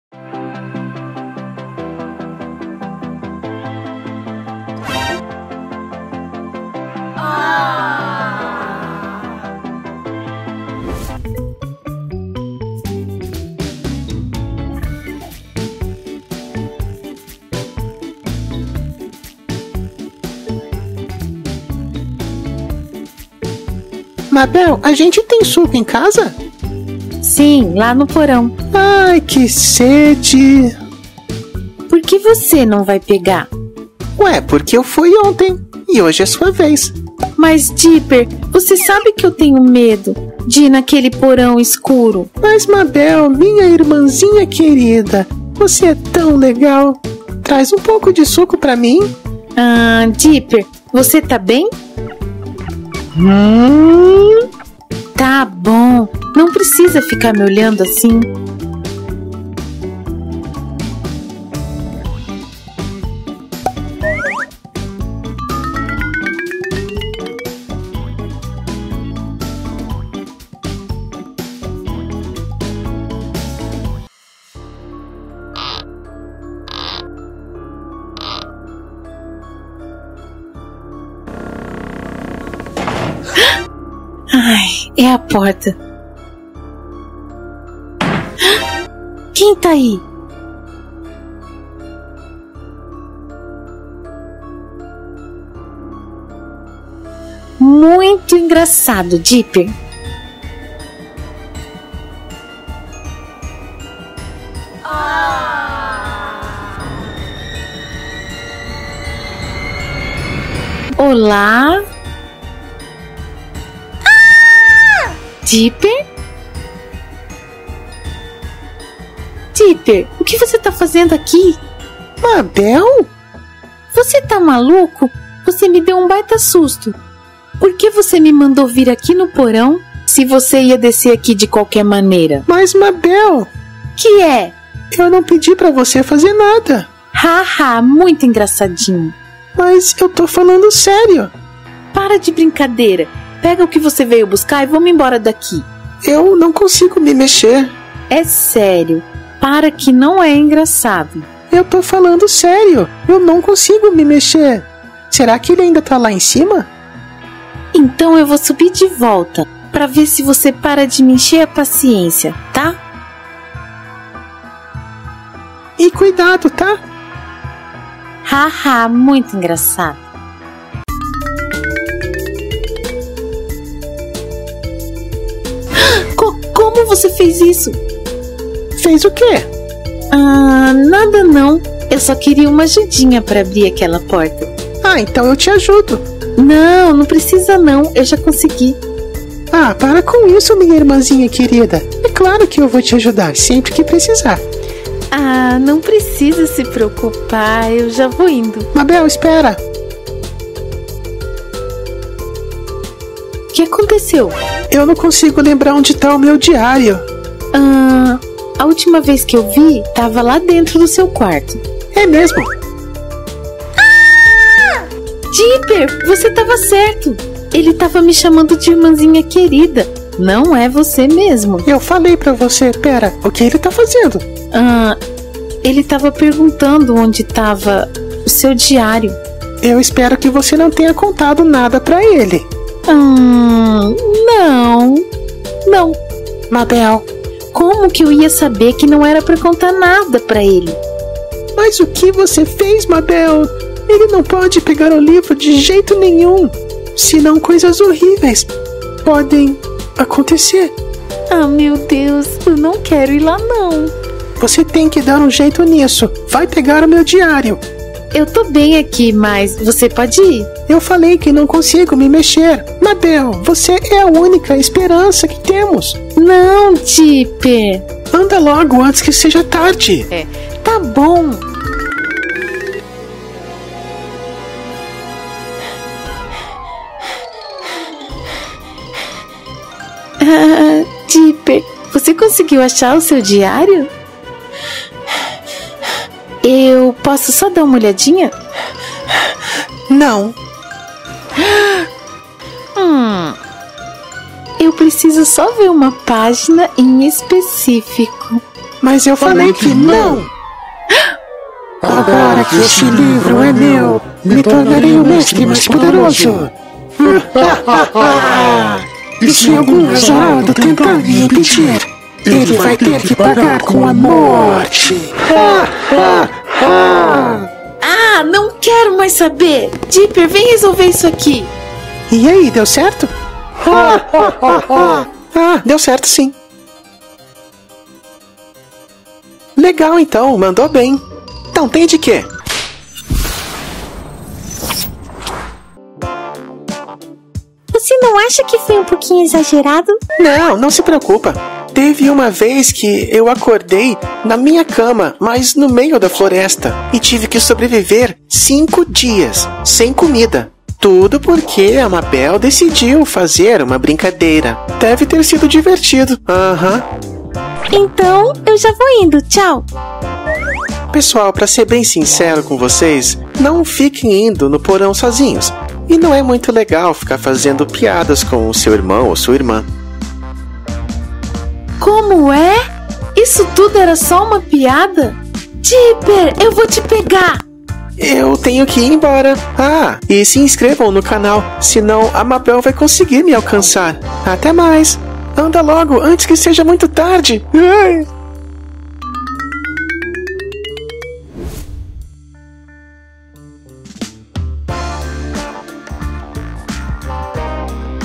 Dipper tem um duplo do mal😱 A irmã resgata seu irmão
Dipper tem um duplo do mal. A irmã resgata seu irmão. Ela será capaz de fazer isso? Ou talvez isso seja apenas uma piada?  Veja a verdade😉

Subscreva o canal para não perder novos episódios.
Clique 🔔 para não perder um novo episódio

Amo todos 💗, tchau a todos!